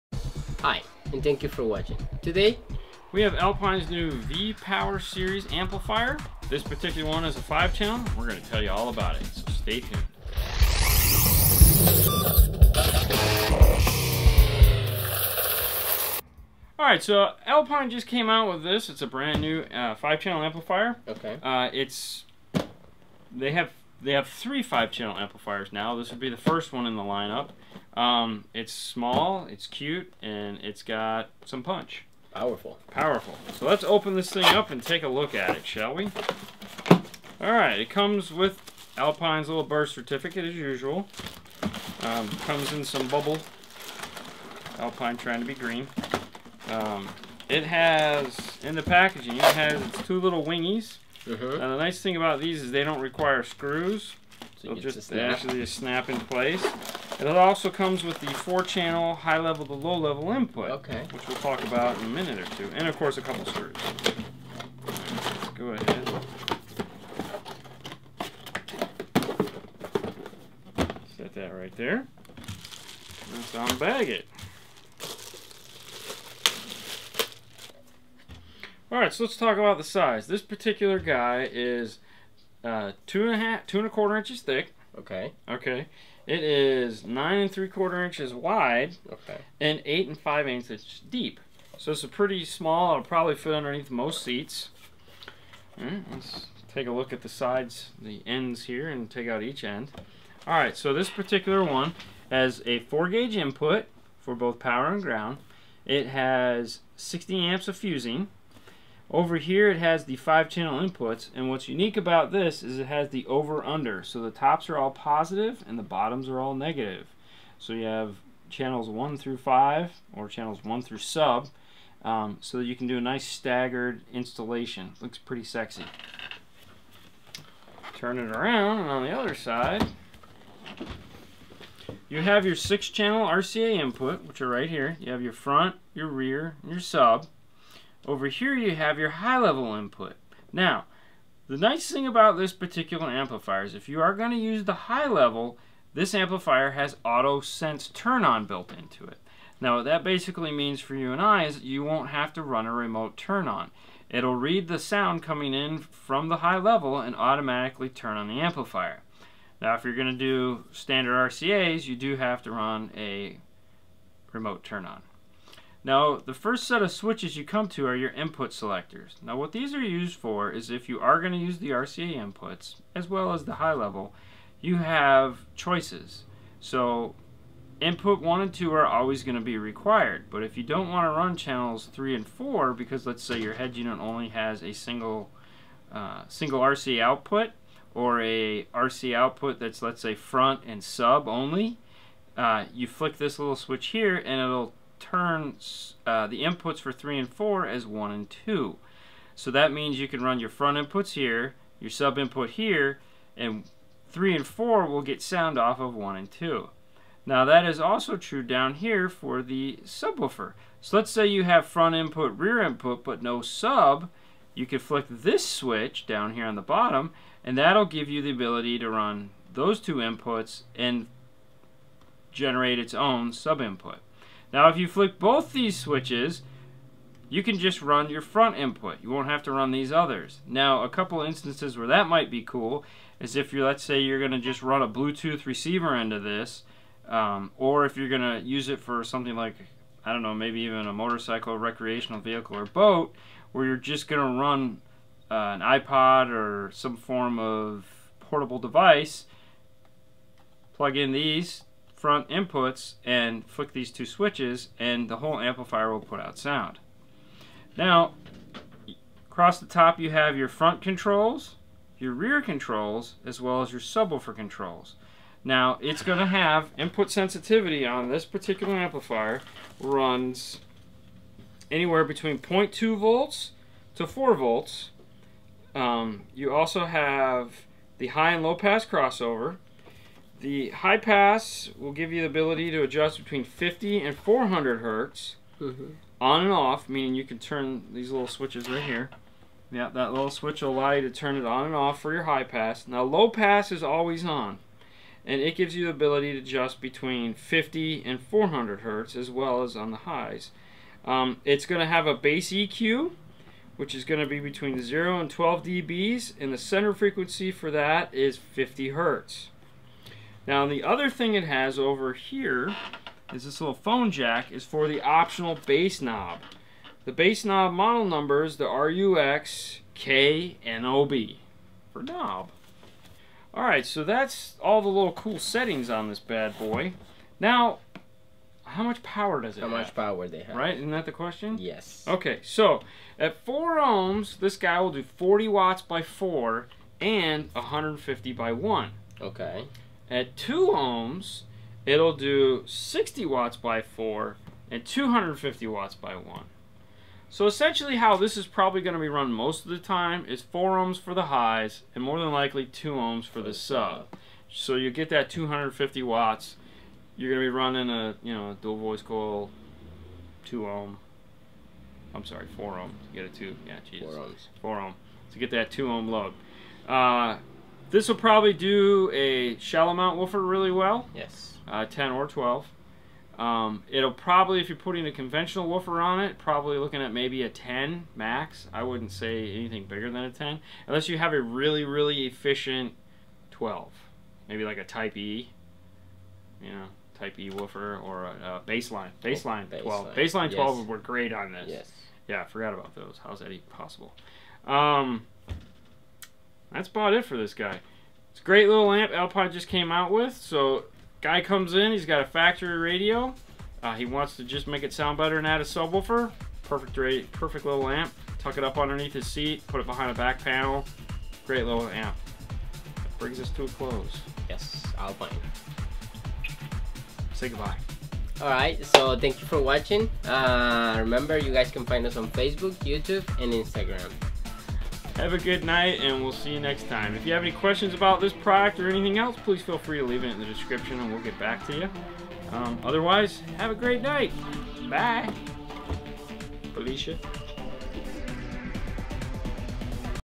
Alpine car audio's new 5 channel amp the MRV V500 you
The new V-Power products are now Class D digital amplifiers, resulting in a huge efficiency gain versus their predecessors without draining your battery or your pockets. Class-D provides continuous clean power to your speakers, operating at 80% efficiency far greater than conventional Class A or B amplifiers. Translation: Enjoy cleaner bass, even during extreme power demandsV-Power amplifiers are known for their compact size, but don’t underestimate their power capacity. Although they have a 40% smaller footprint than their predecessors, they produce the same amount of power as before. Get the power you need, even if you have limited installation spaceOverview
60W x 4 + 250W x1 RMS (2Ω, 14V, ≤1% THD+N)
40W x 4 + 150W x1 RMS (4Ω, 14.4V, ≤1% THD+N)
Same power in a 40% smaller chassis
Evolution to Class D digital platform
New snap-on terminal covers for easy installation
Each V-Power amplifier comes with its very own birth-sheet. V-Power amplifiers are tested and documented to show their true actual output.The V-Power's 4-channel speaker level inputs connect directly to any head unit, without the need for external converters. This makes the V-Power amplifiers ideal for upgrading factory and aftermarket systems. The flush mount controls make for easy installation. The terminal covers conveniently snap-on without the need for additional tools, to protect the wiring connections while creating a clean, finished appearance